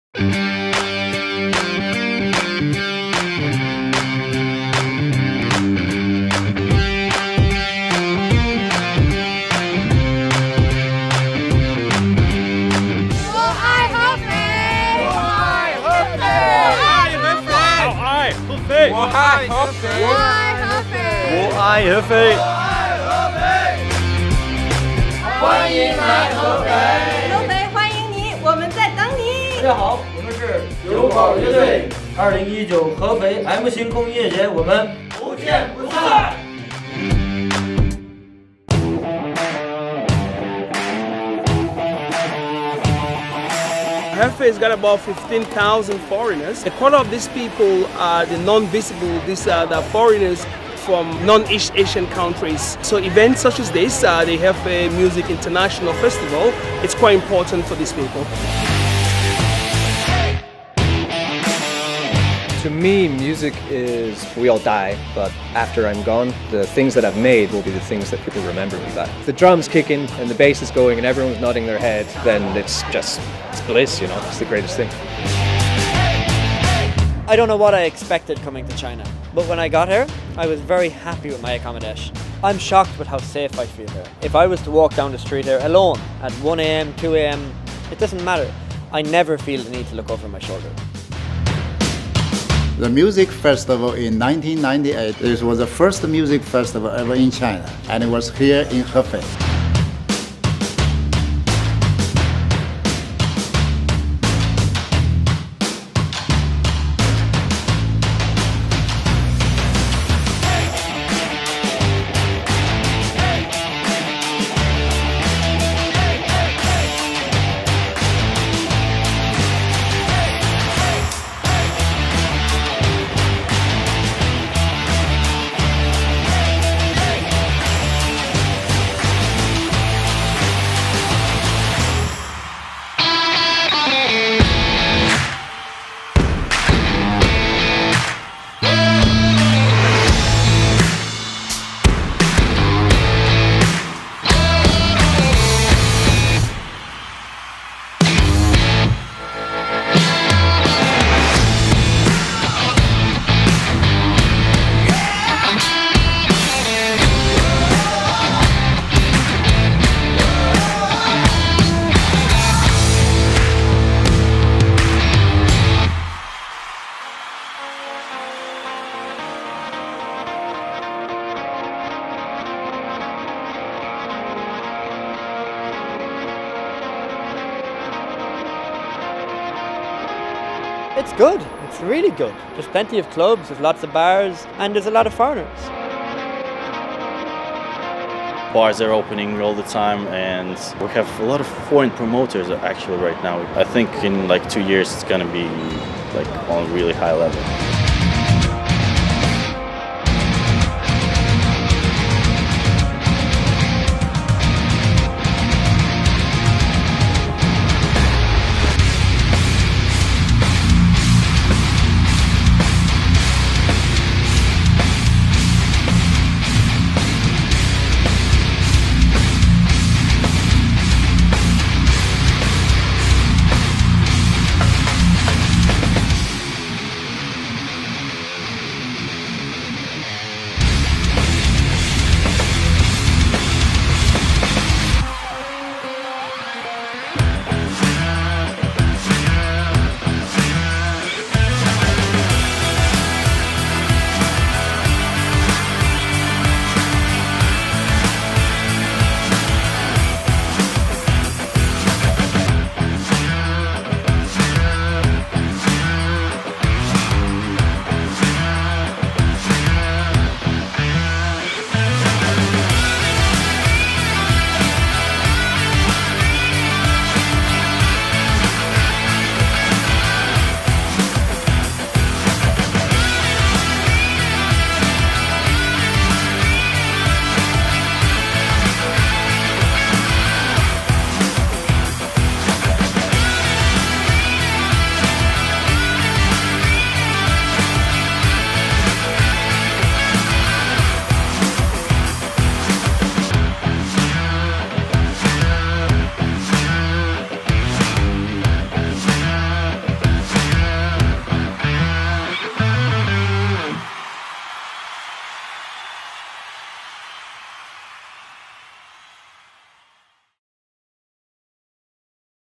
M+ International Music Festival Official Aftermovie | Hefei, China
I was hired to film an international music festival in Hefei, China. Here is my rendition of the final product for the event!

The official statement from the festival director:

To commemorate the 40th anniversary of China's Open Door policy,  and to highlight the great achievements and internationalization of Hefei's developments, the 2018 Hefei M+ Music Carnival and German Culture Week was successfully held at the Anhui Sports Center from November 29th to December 5th, 2018.

M+ is also the 20th anniversary of the first Chinese music festival,
held in Mingzhu Square, Hefei in 1998.

为纪念中国改革开放40周年，彰显合肥城市发展取得的巨大成就以及国际化进程，2018合肥M+星空音乐嘉年华暨德国文化周于2018年11月29日-12月5日在安徽省体育中心成功举办.

同时，M+，也是1998年在合肥明珠广场举办的中国第一个音乐节20周年纪念.